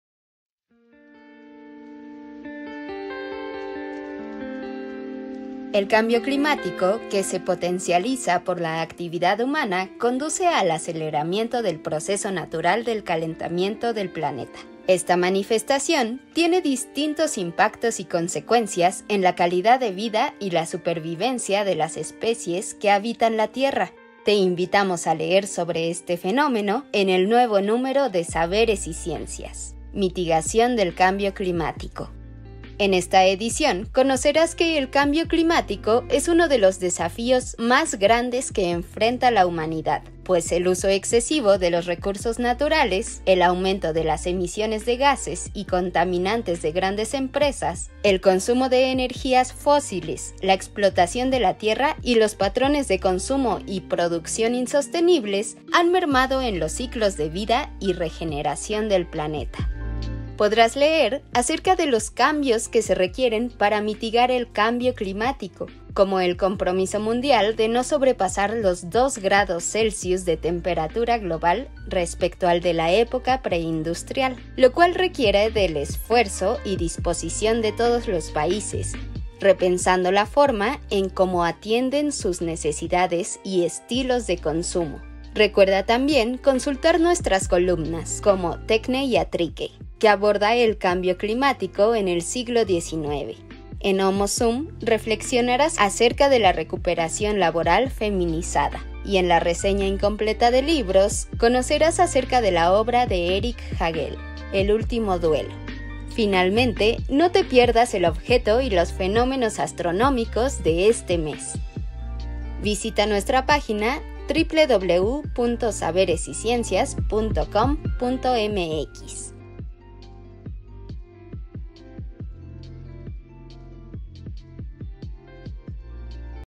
Te invitamos a leer el nuevo número de Saberes y Ciencias: Mitigación del cambio climático
Te invitamos a leer el nuevo número de Saberes y Ciencias: Mitigación del cambio climático, en el que podrás conocer los esfuerzos y alternativas que buscan amortiguar el aceleramiento del calentamiento global